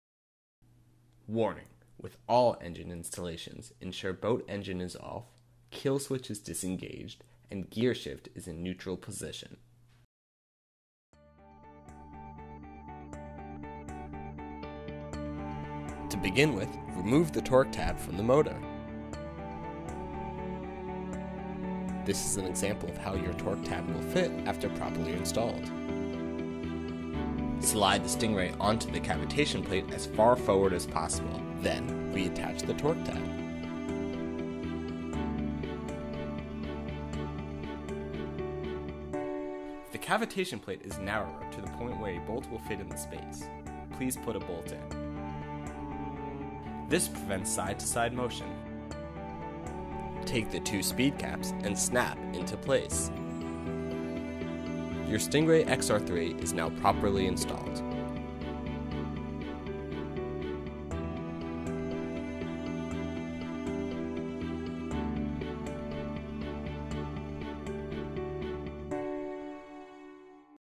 StingRay XRIII Installation
StingRay XRIII Installation Instructions:

WARNING: WITH ALL ENGINE INSTALLATIONS, ENSURE BOAT ENGINE IS OFF, KILL SWITCH IS DISENGAGED, AND GEAR SHIFT IS IN NEUTRAL POSITION.

1.    Make sure all installation hardware installation hardware is included: two (2) bolts, two (2) self-locking nuts, and two (2) speed caps.

2.    Remove the torque tab from the underneath the cavitation plate.

3.    Slide the StingRay XRIII onto the cavitation plate as far forward as possible.

4.    Reattach the torque tab with the StingRay XRIII still in place so that the torque tab clamps the hydrofoil in place.

5.    If the hydrofoil can be moved from side to side on the cavitation plate, insert the two (2) bolts from the bottom up through the slots in the front of the hydrofoil. There is no need to drill through the cavitation plate! These bolts are only used to eliminate the side to side play of the installed hydrofoil.

6.    Use the two (2) self-locking nuts to attach the bolts. The white side of the nuts should be facing upwards.

7.    Use a Phillips screwdriver to tighten the bolts. After bolts are tightened, place the two (2) speed caps into the two slots to cover up the attached nuts and bolts. In a matter of minutes you're ready to enjoy unmatched performance!

Notice:

(1) This product should make an immediate performance improvement in your boat/motor...if for ANY REASON it does not, then remove this product and call for assistance.
(2)  Any surface "flow marks" around holes that appear to be cracks are a normal part of the molding process and do not affect the part strength or performance.